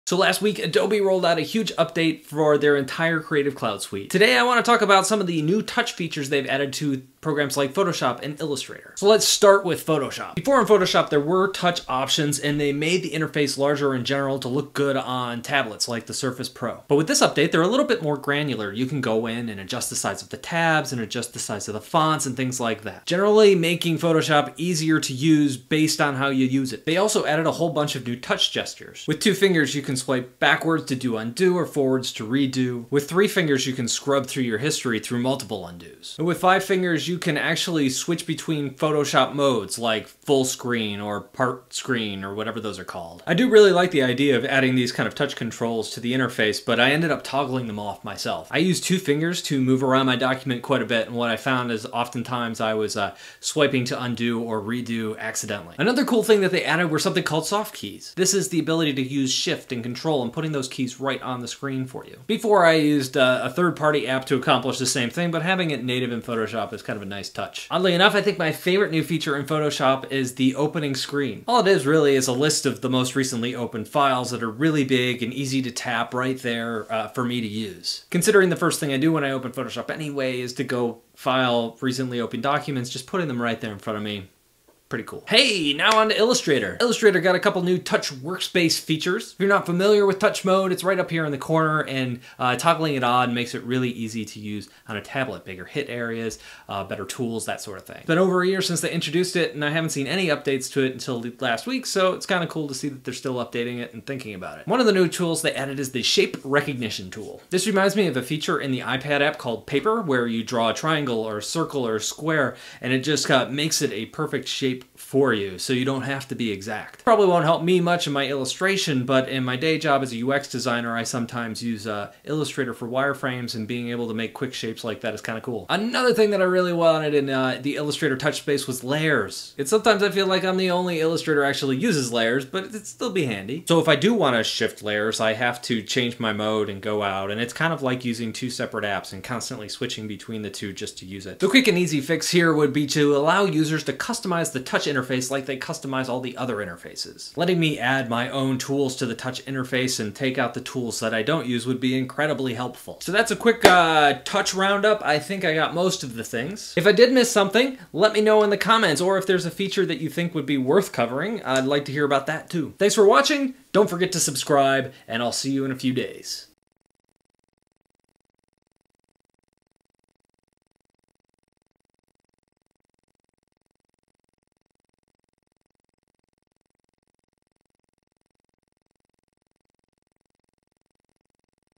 Photoshop and Illustrator updates for the Surface Pro
Adobe Rolled out a bunch of new features to their Creative Cloud Apps last week. Here is the overview of some of the specific touch enabled tools they included for Windows tablets like the Surface Pro.

Wonder what tools I draw with or what I use to make videos, check out my Gear